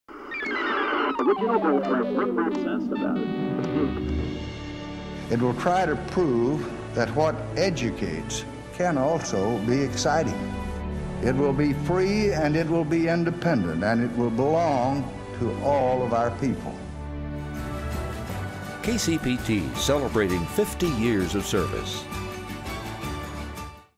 KCPT 50 Years of Service: 1967
In 1967, President Lyndon B. Johnson signs the Public Broadcasting Act into law.